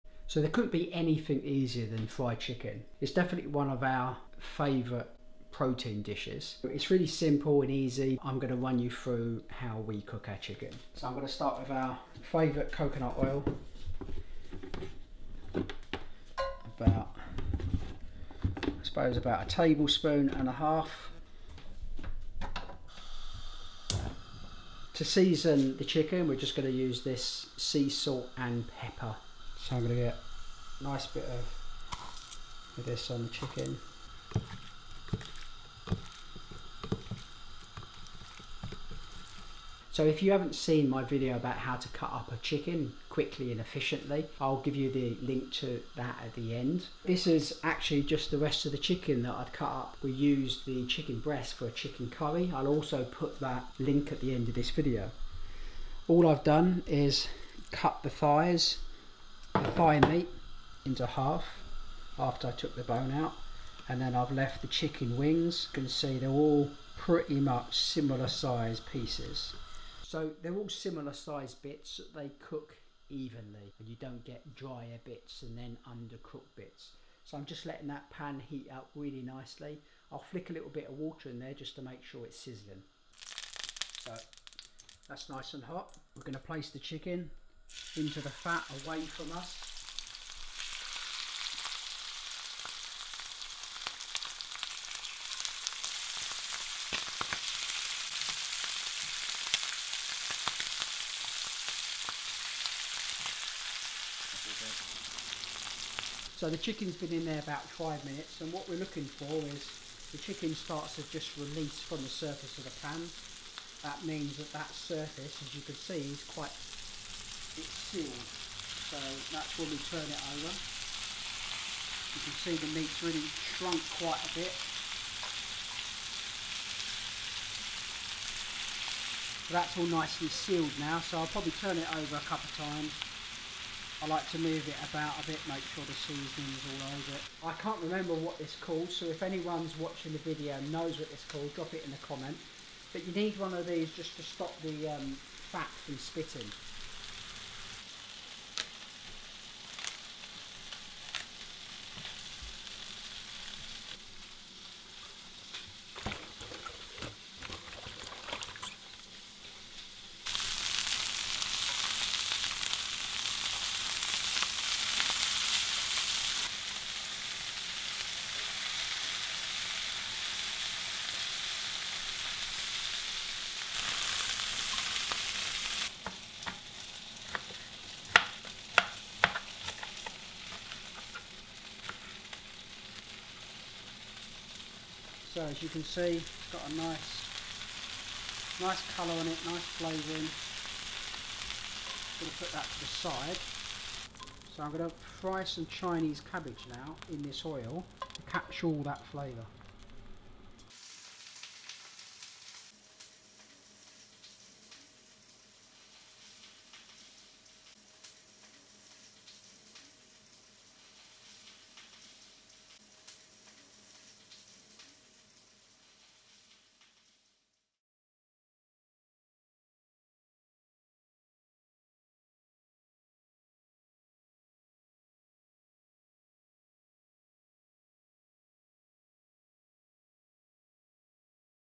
Fried Chicken
Easy quick-fried Chicken, healthier because it's fried in organic coconut oil and not vegetable oil.

Chicken, thighs ( deboned ) and chicken wings.

And a vegetable like Chinese cabbage or regular white/red cabbage, Courgettes, carrots or squash to soak up the chicken juices.